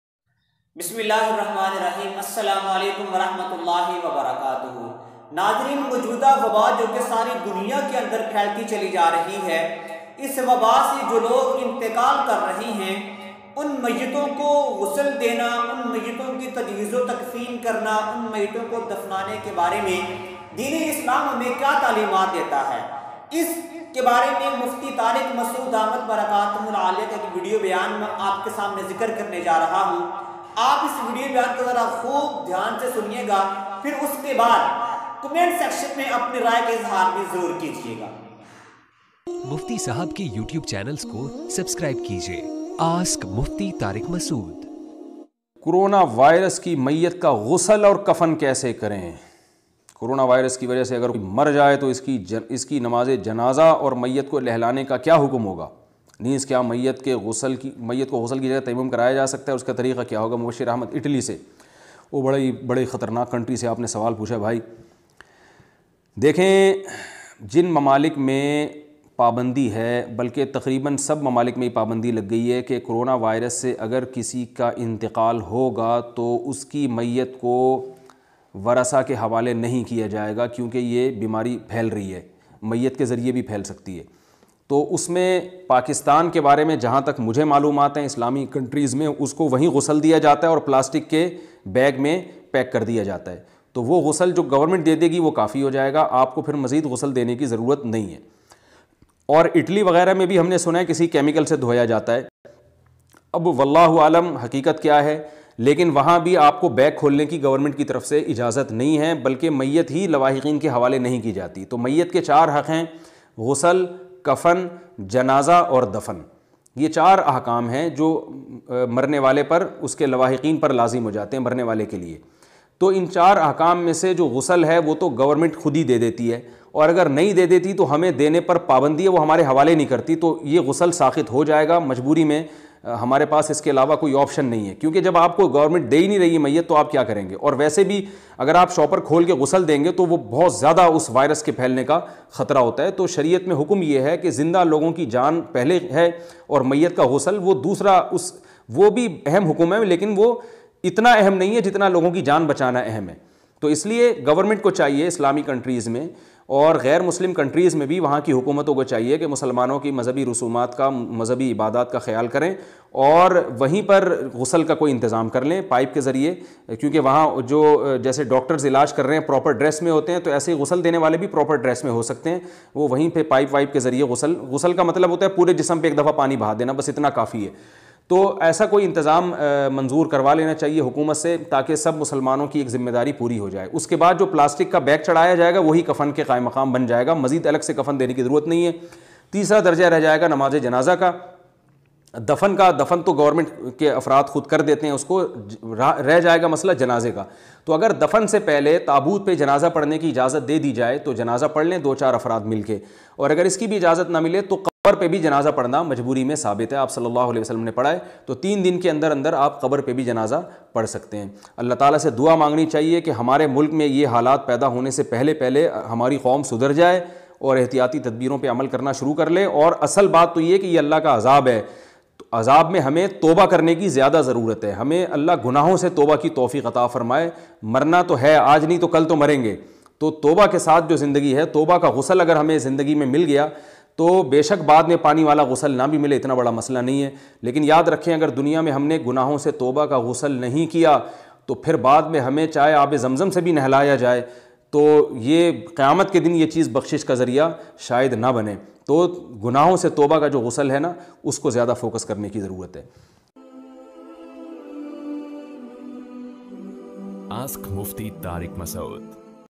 Mojoda Waba Me Mayyat k Gusal wa Janaza Ka Hukam| Problem Solved
is video me apko btaya gya h Gusal or Janaza Ka hukam .tafseel k sath by Reaction video HM ASIF OFFICIAL